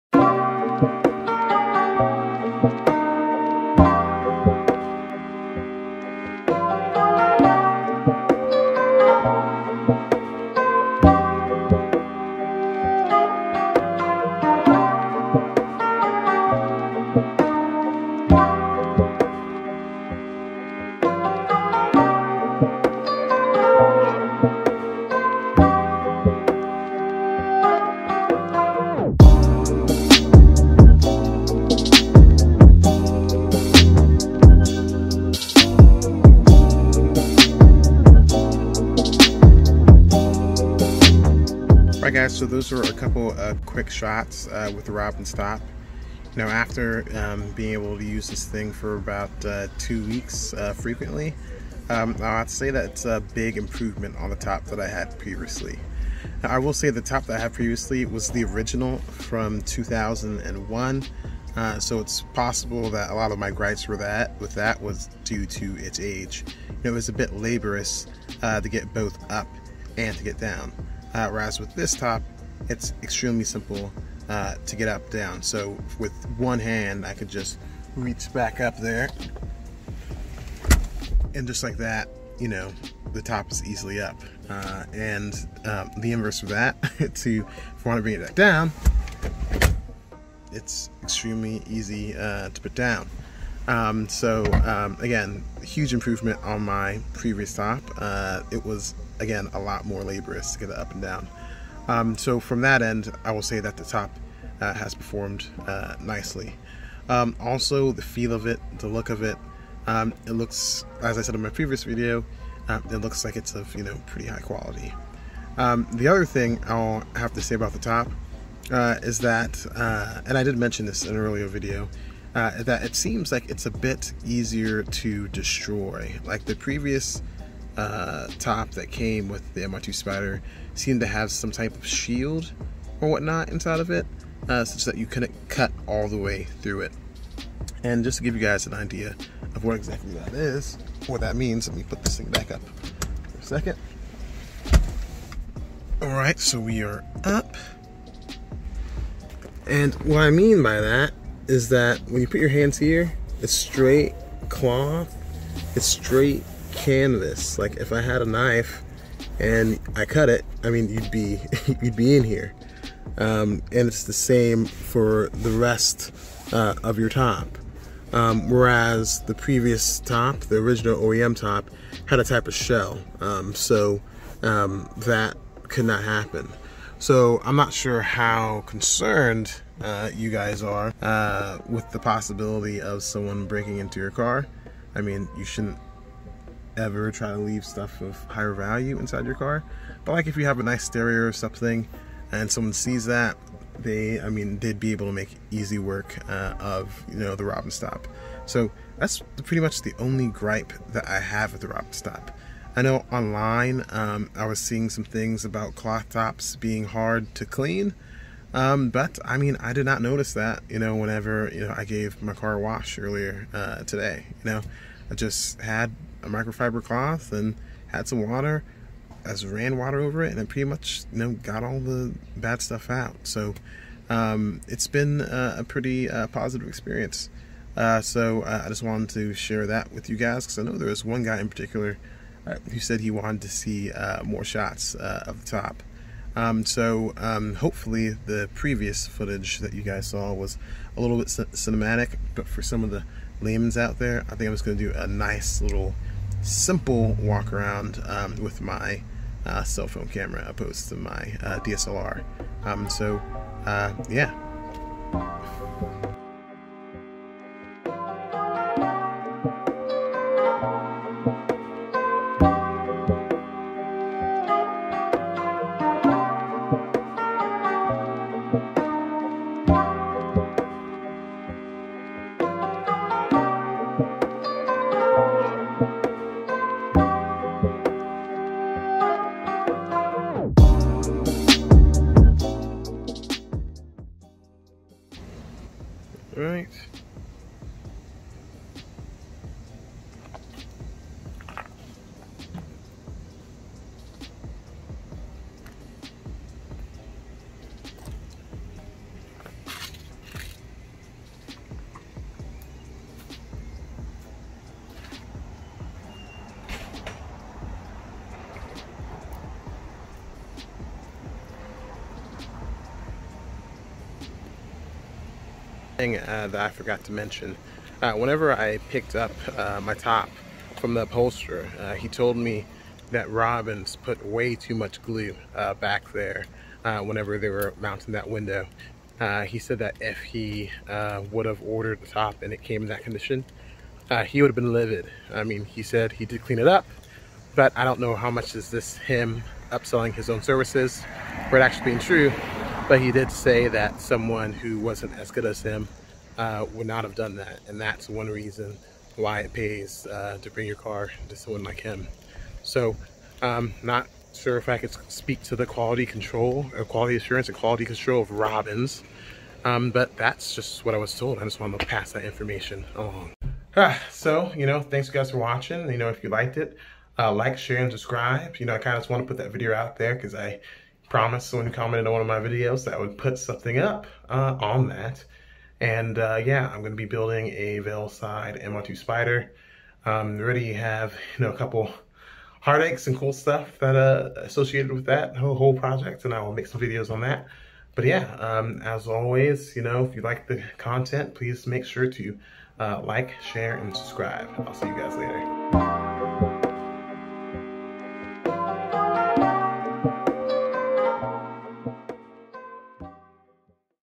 Robbins Soft Top (Review/Impressions) - Toyota MR2 Spyder
First impression of the Robbins top for the Toyota MR2 Spyder.